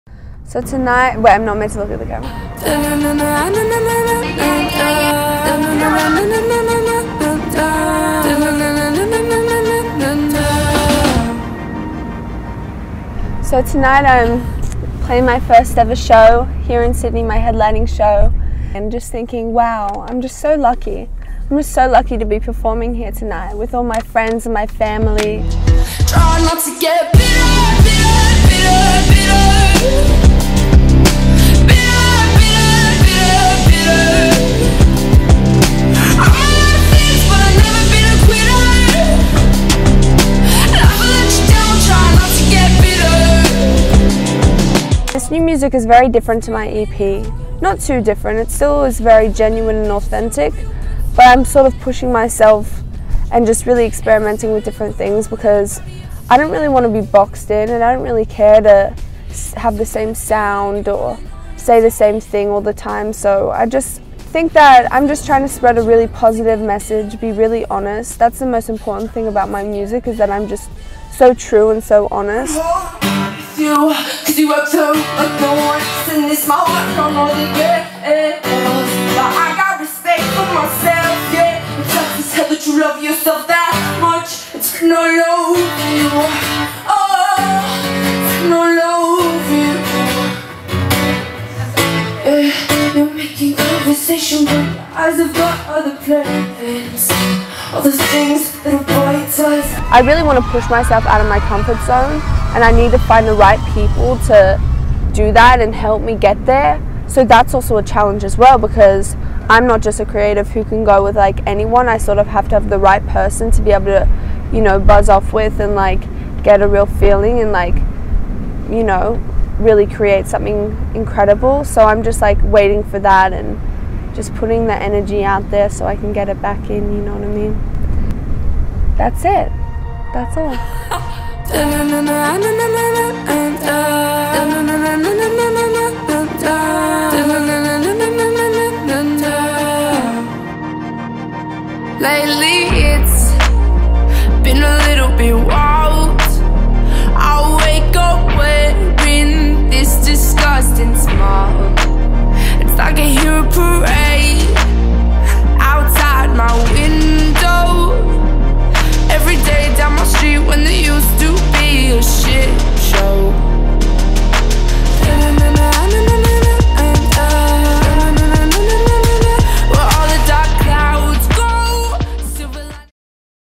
Evie Irie - Homecoming Debut Showcase - Sydney, Australia
I played my first ever debut showcase at home in Sydney with all of my family and friends around me. I went for the crowd surf... watch to see if I survived!